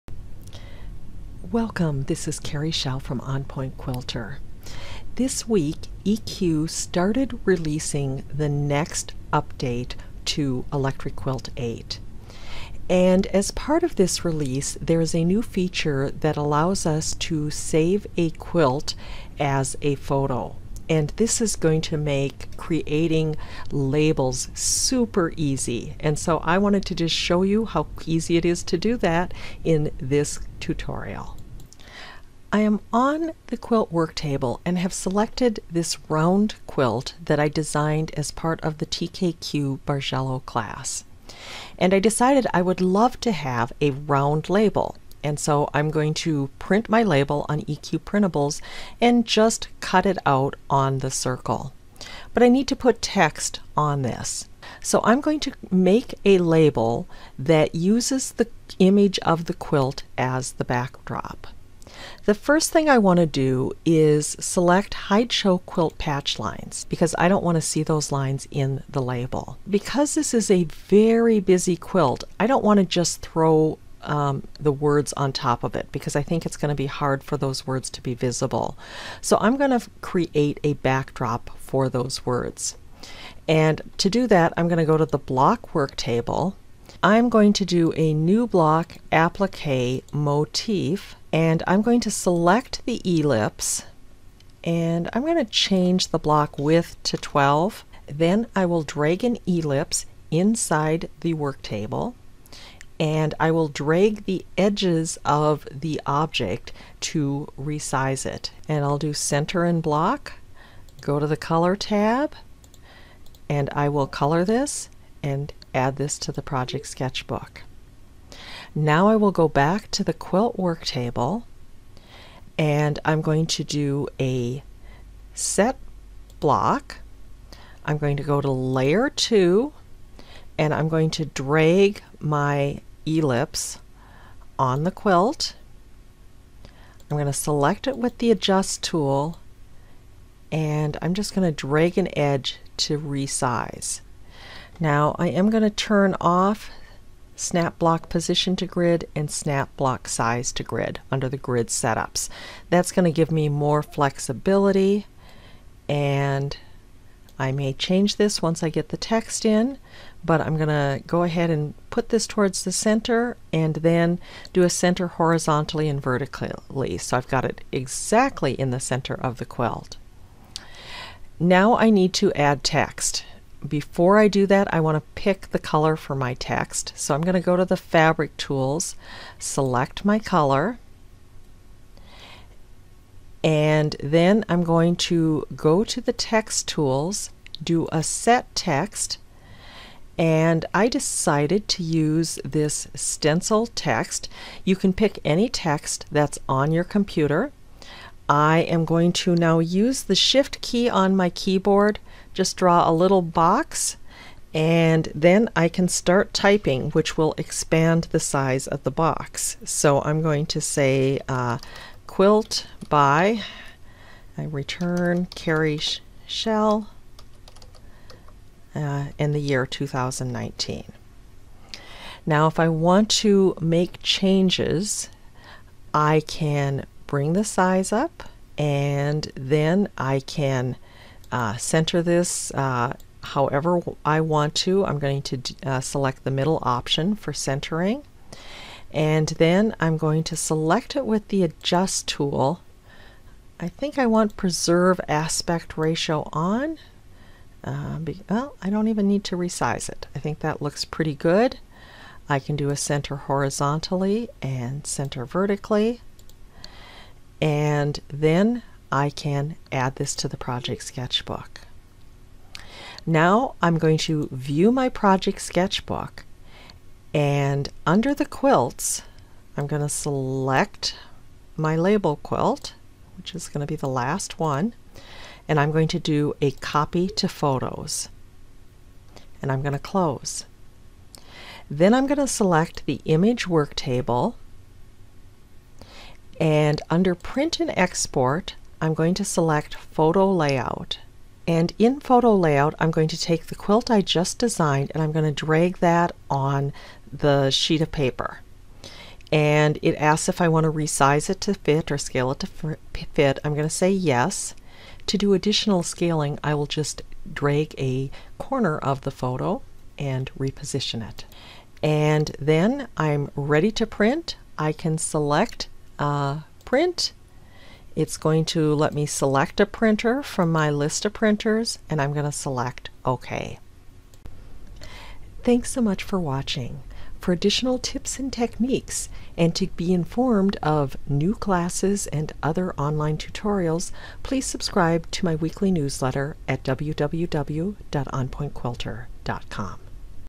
Easy Quilt Labels - An Electric Quilt 8 Tutorial
In this EQ8 tutorial, learn a quick way to create and print quilt labels using your designed quilt as a backdrop.  It is now much more user friendly.